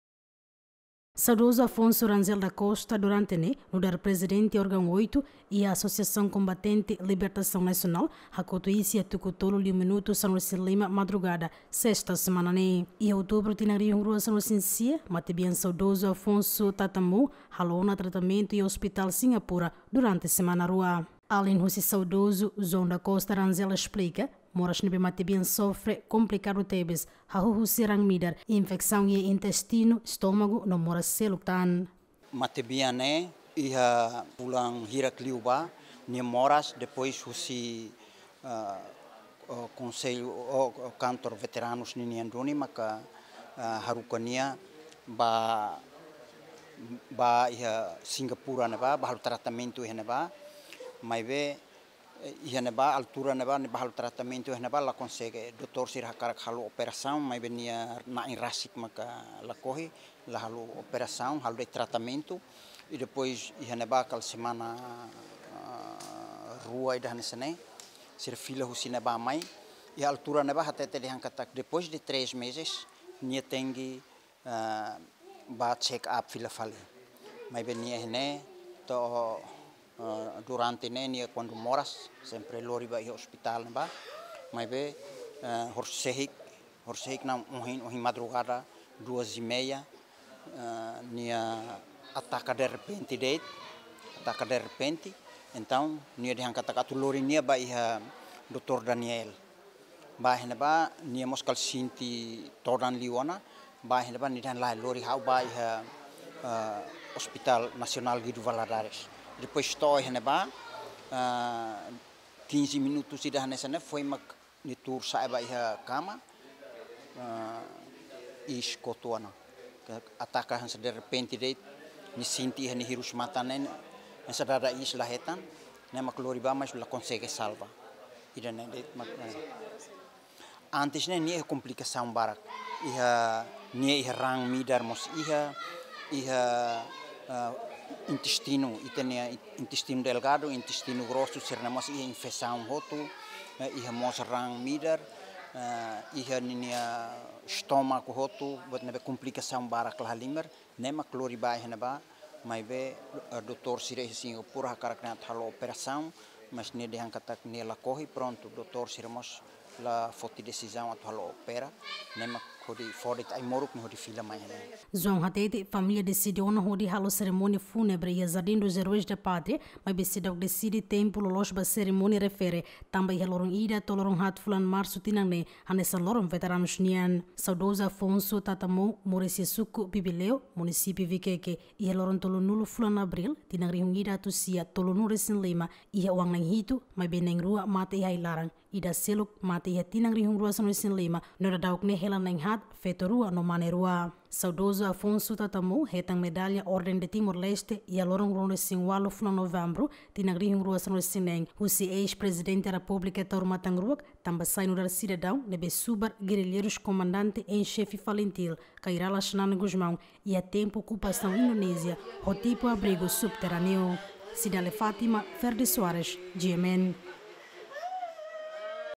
Veteranu Afonso Rangel da Costa ho naran kódigu Tatamou hakotu iss
Veteranu Afonso Rangel da Costa  ho naran kódigu Tatamou hakotu iss iha sala emerjensia Óspital Nasionál